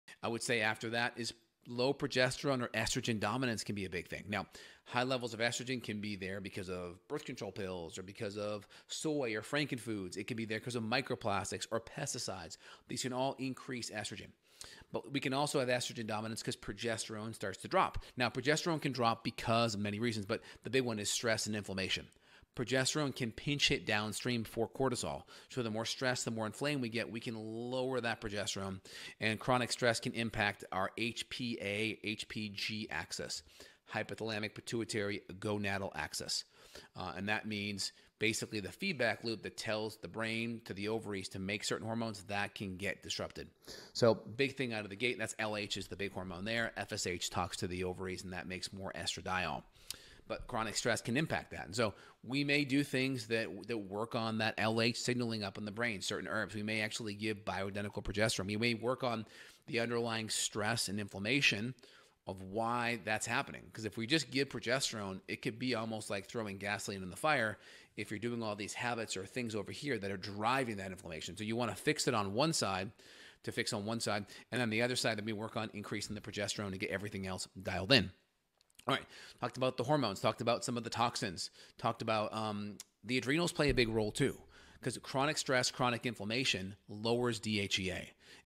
How Stress Destroys Your Hormones
Ever feel like your hormones are all over the place? You’re not crazy—low progesterone and estrogen dominance might be the real culprits. Chronic stress can trigger inflammation that lowers progesterone and disrupts your HPA axis. That’s where hormonal chaos begins. Let’s talk about how stress-related hormone imbalances are impacting your mood, energy, and cycles. These shifts can also be tied to everyday exposures like microplastics and pesticides in food, which spike estrogen unnaturally.